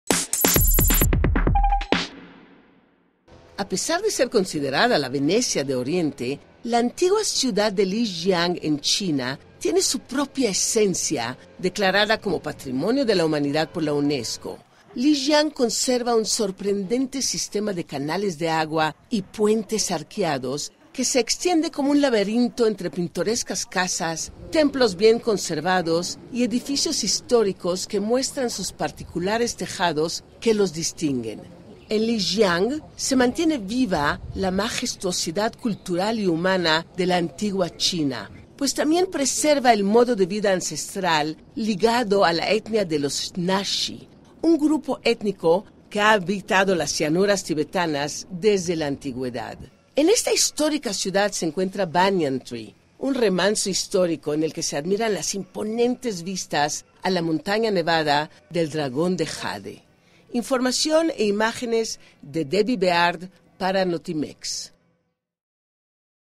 La Venecia de Oriente
Declarada Patrimonio de la Humanidad por la UNESCO en 1997, Lijiang mantiene con vida toda la majestuosidad de la antigua China.